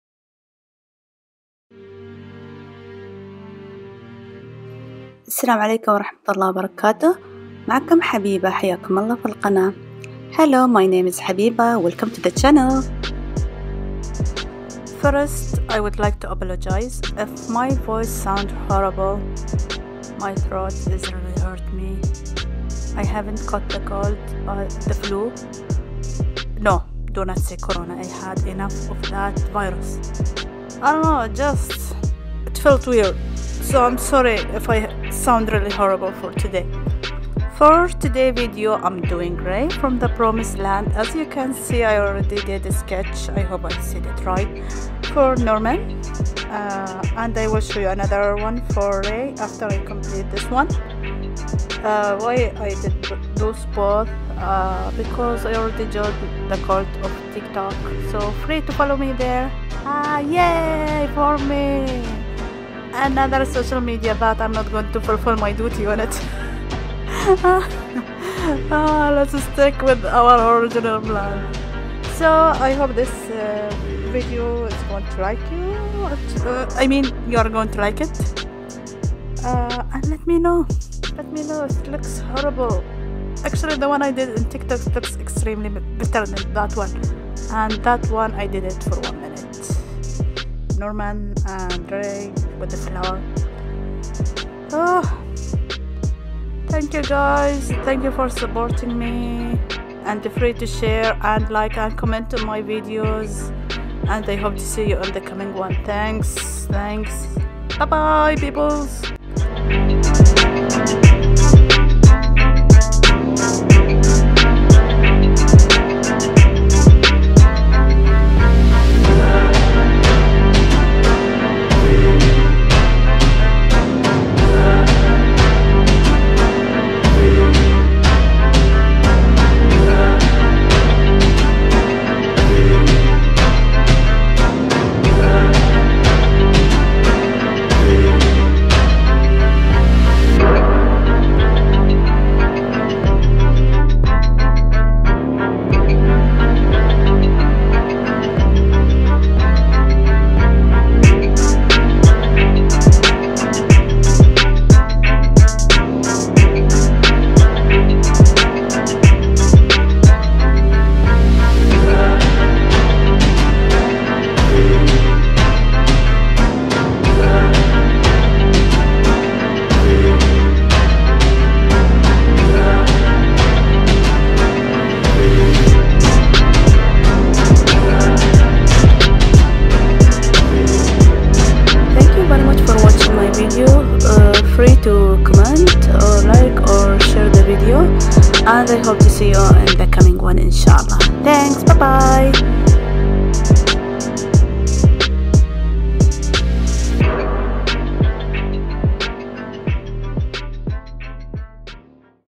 【The Promised Neverland】 Ray sketch
doing sketches for ray from The Promised Neverland

Don’t forget to push the like button ..

Suggestion : Free to ask for any tutorials or Anime Characters to draw .

Tags to followed